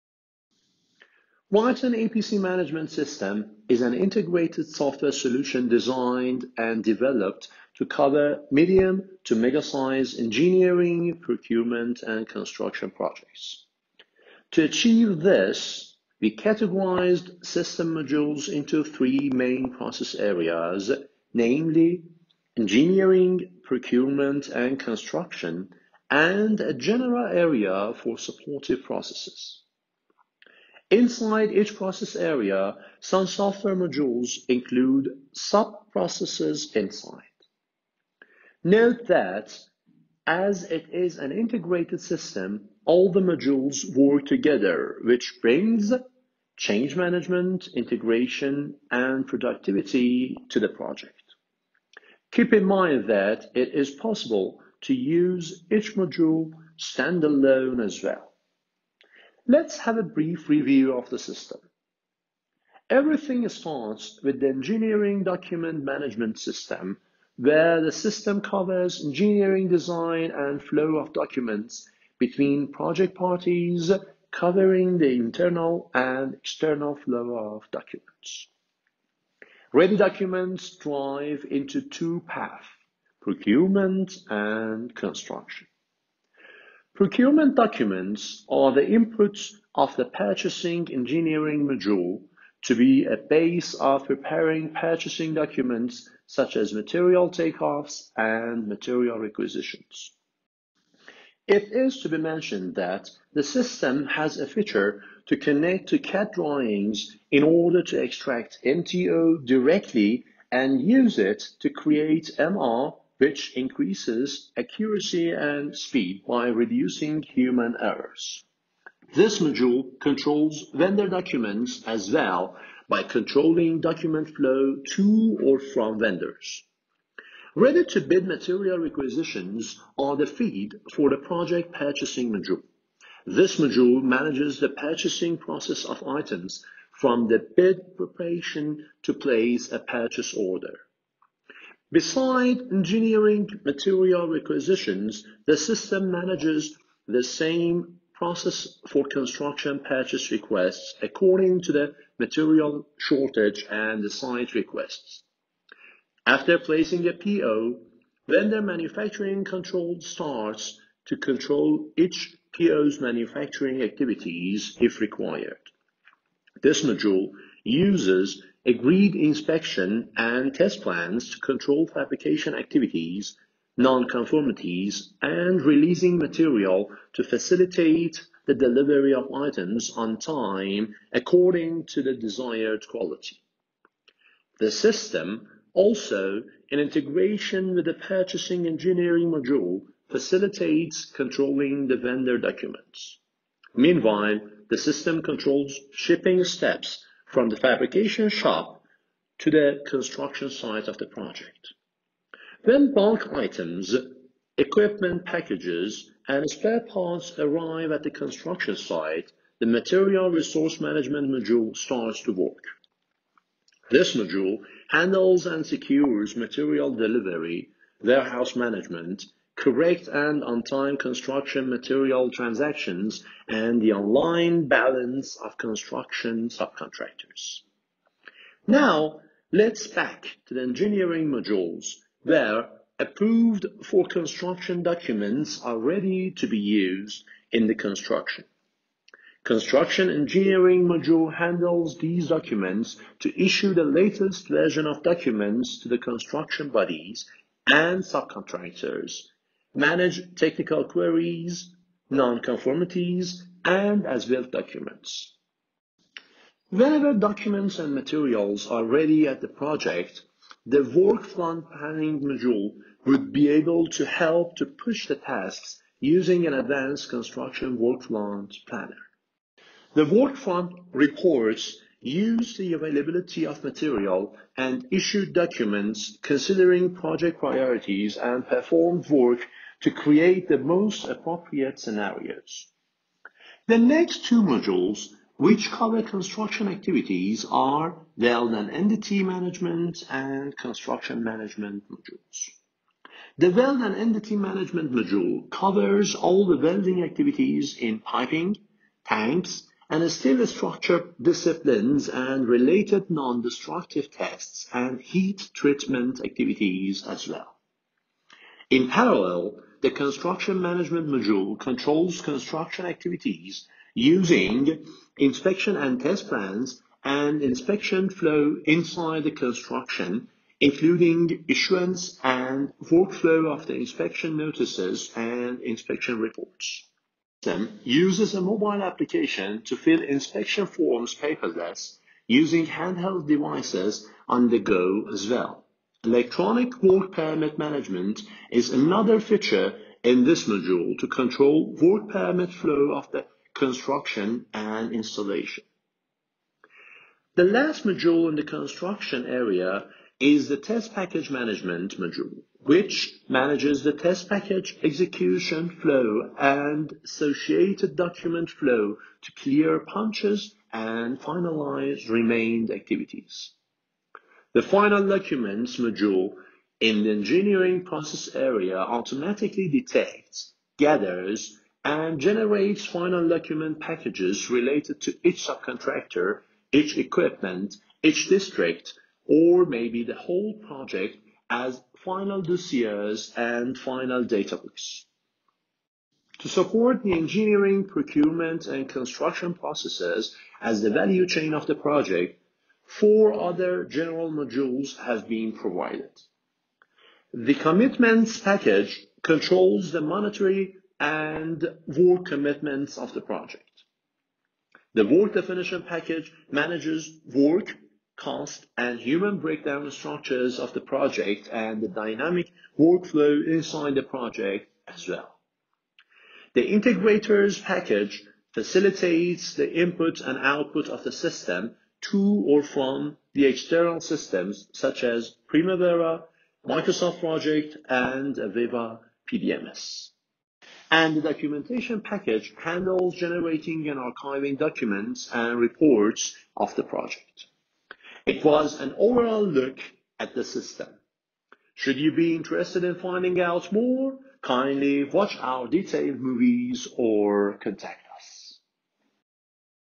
Rhyton EPC Management software introduction
Rhyton EPC management system is an integrated software solution designed and developed to cover medium to mega-size Engineering, Procurement, and Construction projects.
To achieve this, we categorized system modules into three main process areas, namely Engineering, Procurement, and Construction, and a General area for supportive processes.
Inside each process area, some software modules include sub-processes inside.
Note that as it is an integrated system, all the modules work together, which brings change management, integration, and productivity to the project.
Keep in mind that, it is possible to use each module stand-alone as well.